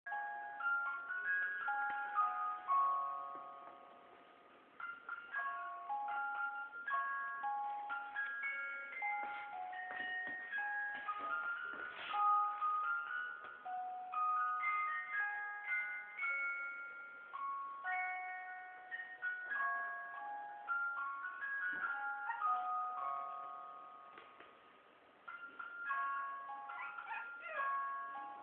Maltesi-Micro cucciolo di maltese, primi passi - First step of maltesian puppy. 20 days
Sneaky è un cucciolo di maltese, ha 20 giorni e impara a camminare ma la retro non gli riesce molto bene,  Ma come fa ridere !

Sneaky is a Maltese puppy, he has 20 days and learns to walk but the back does not do very well, But how does he laugh!